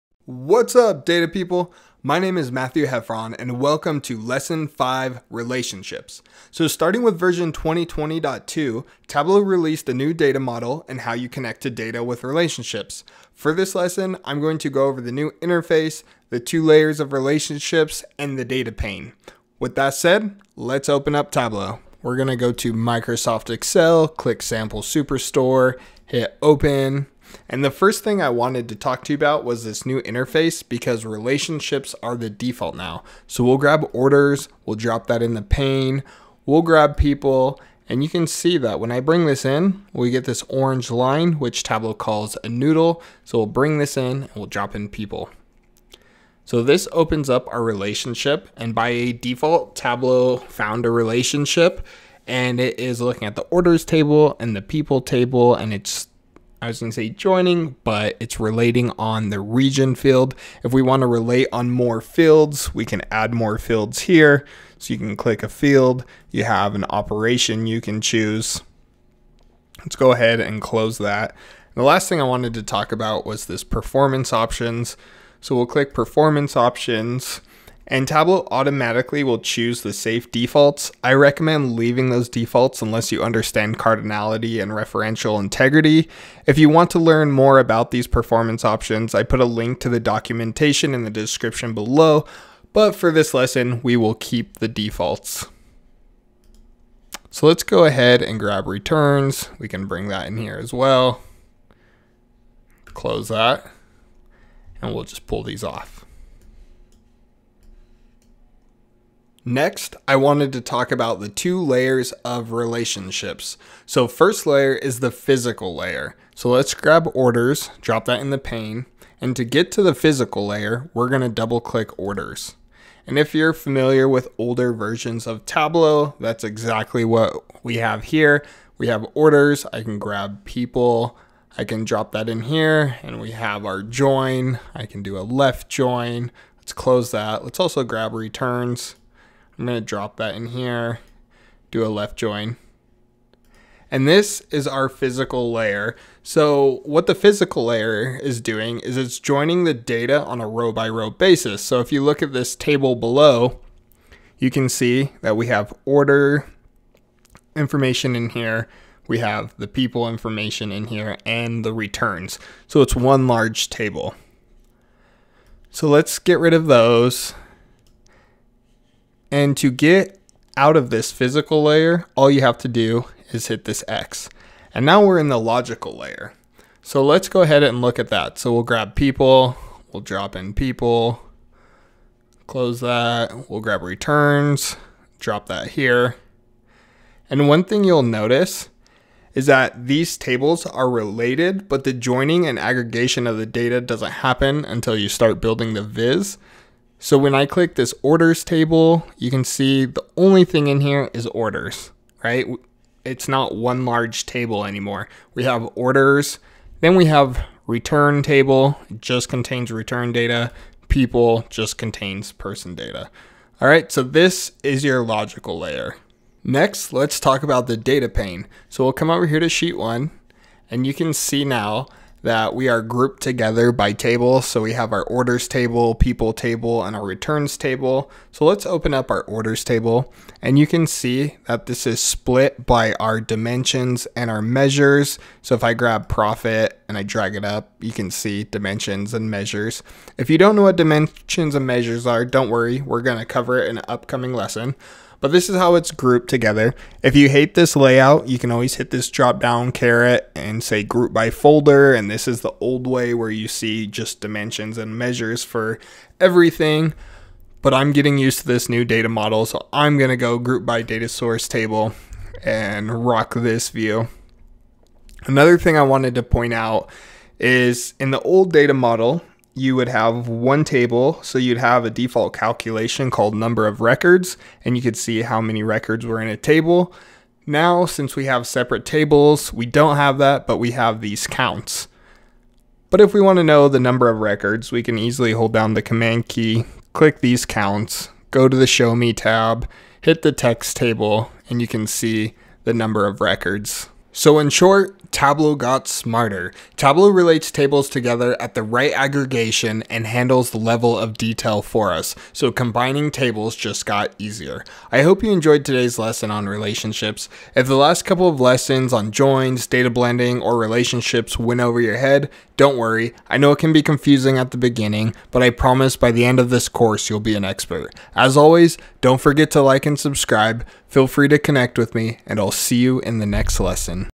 Lesson 5 - Relationships
In this lesson, I go over Tableau's new Data Model and discuss Relationships.